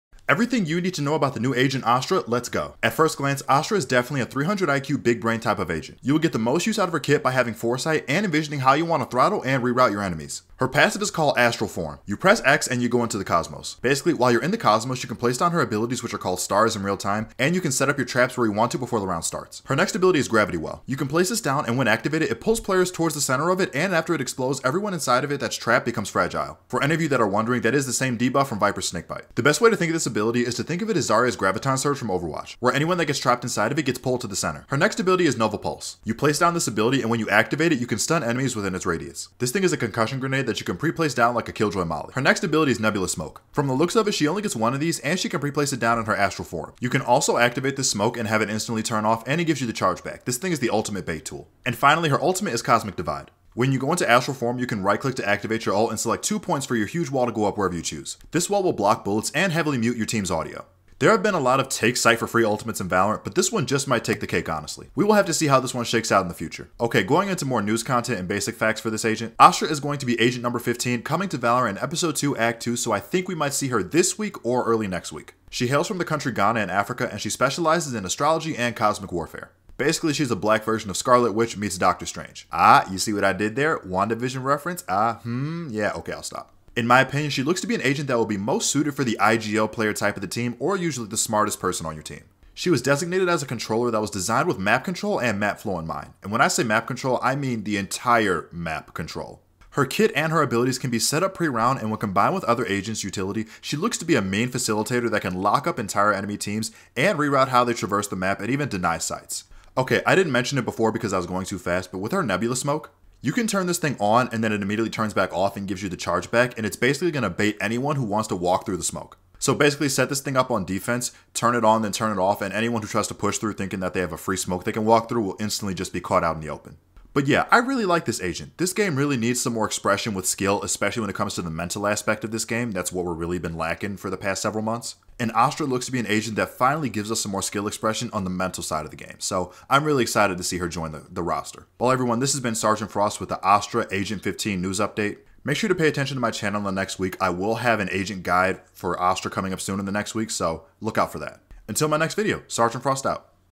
Valorant: ASTRA ABILITIES EXPLAINED! (Agent 15 leaks and reveals)Valorant
A quick Valorant news breakdown and explanation of the new Ghana Agent 15 Astra. i explain what her abilities do and what type of agent i think she will become in the future. Keep an eye out on my channel for my Astra guide coming soon.